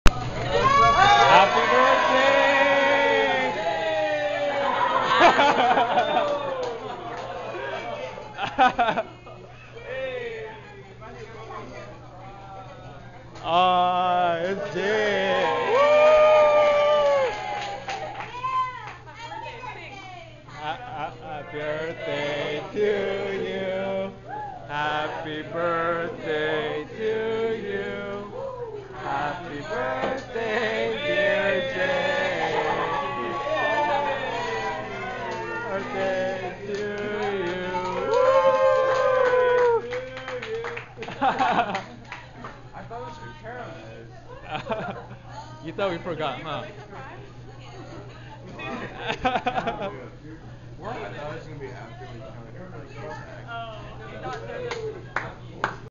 Surprise Jae!
Surprising Jae for his (early) 19th b-day at Korean bbq restaurant